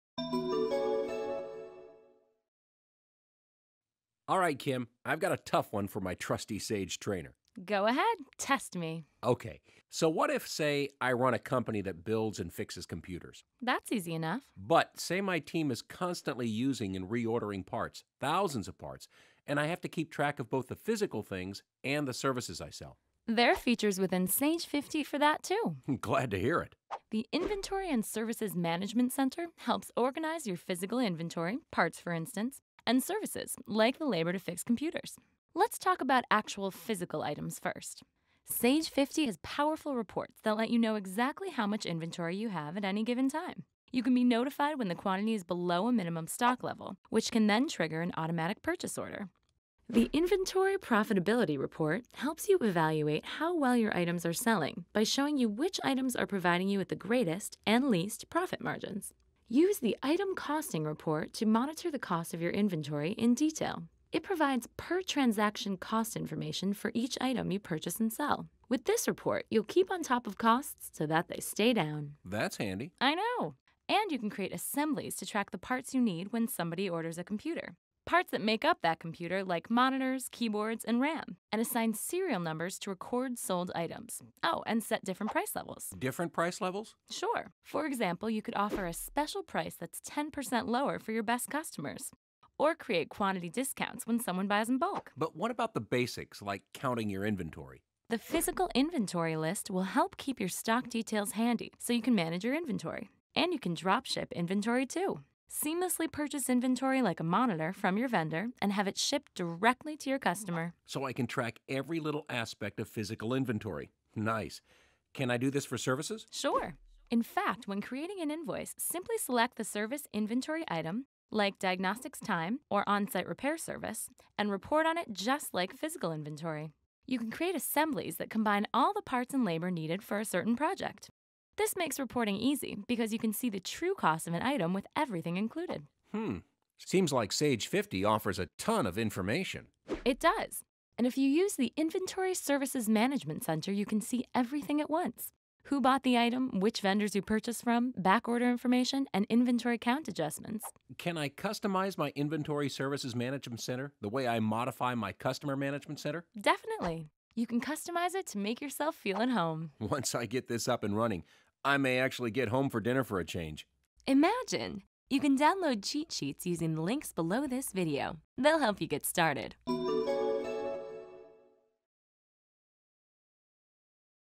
Managing Inventory in Sage 50.
Sage 50's Inventory & Services Management Center helps organize your physical inventory and services through tracking and reports, building assemblies, setting prices, tracking serialized inventory items, and more.